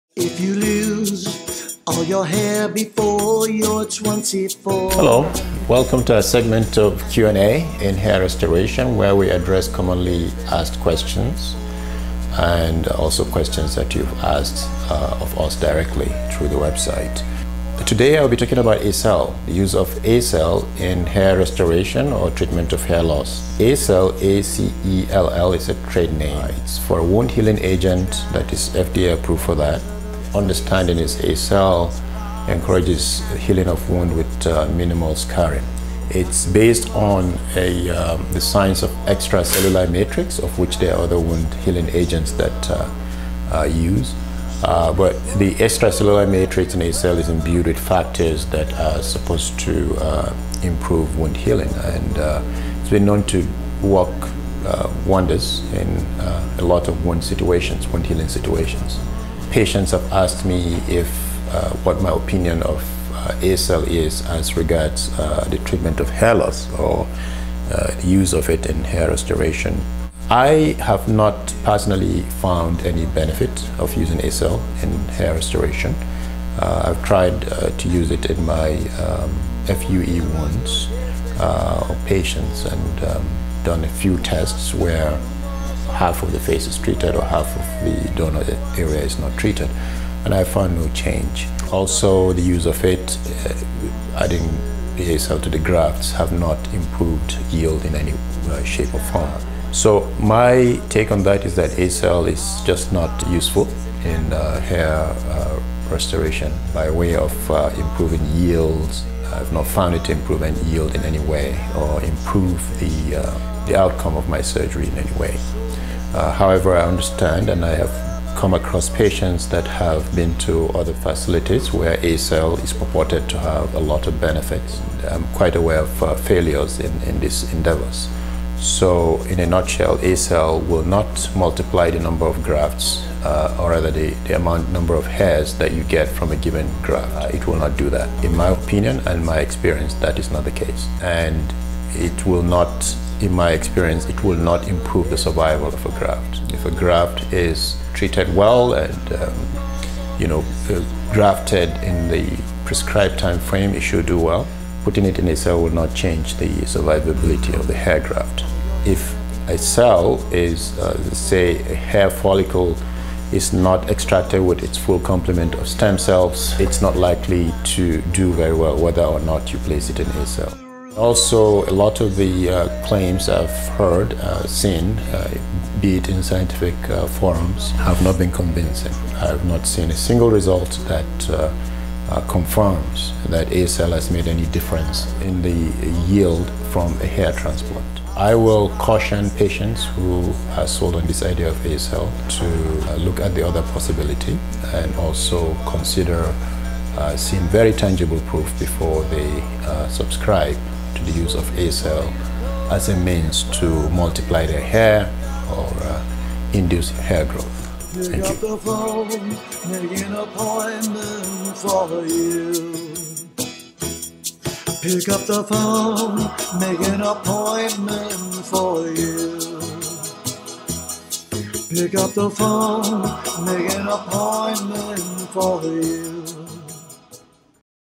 ACELL: Is it useful in hair restoration?
Many individuals who are considering a hair transplant surgery wonder if they should choose an additional treatment called ACell to achieve the best possible growth. In this video, Dr. U provides his insight in response to this question.

ACell is a bioengineered product made from the extracellular matrix of a porcine bladder along with other factors that supposedly facilitate new cell growth.  It has been FDA approved for wound healing purposes.

ACell serves as a scaffold which encourages the regeneration of tissue. It is often used in various types of reconstructive surgery (e.g. veterinary medicine). And since it is shown to regenerate tissue in certain contexts, some believe that it has the potential to improve growth yield and wound healing following a hair transplant procedure.

In this video, Dr. U explains his view on whether or not ACell is effective for hair restoration. He himself has not been able to reproduce the results claimed by proponents in terms of regenerating follicles and improving wound healing.

Dr. U describes tests that he has performed on donor areas hair transplant patients. He applied ACell to one side of the region, while leaving the other side untreated. And there was no difference in the quality of wound healing on either side.

Dr. U also explains that he tried applying ACell to the hair transplant grafts and found no improvement in the resulting yield. He himself has not found that ACell had improved his surgery in any way.

Also, Dr. U explains that ACell will not improve the survival of grafts. If the follicles had been severed and thus missing some of its stem cells, the use of an external product like ACell would not likely reverse this error. Nor would it help compensate for the selection of hair follicles that have already started down the path of miniaturization.

Furthermore, Dr U notes that he has not seen tangible support for any of the claims made in support of using ACell for improving hair transplant outcomes.

Patients who are curious about the use of ACell for hair restoration should take note of these types of considerations when deciding to opt for this treatment or not.

Dr. Sanusi Umar